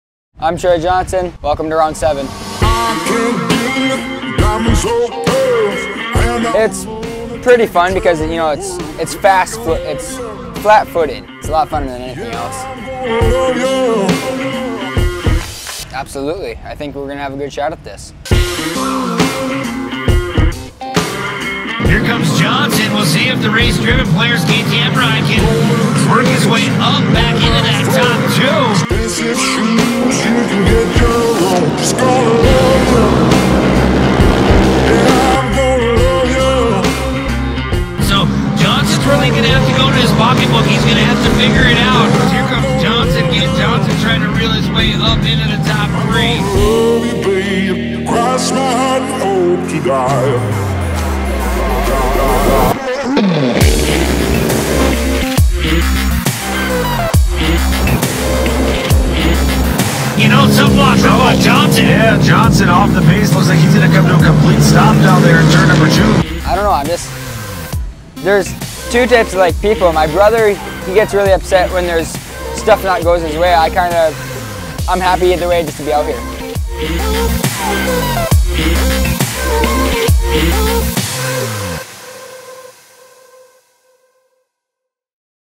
CHAMP OFF ROAD_TROY JOHNSON
From The Big House in Crandon, WI. Mod Cart Driver Troy Johnson gives his insight into the Championship.

A production of SPI Media Core LLC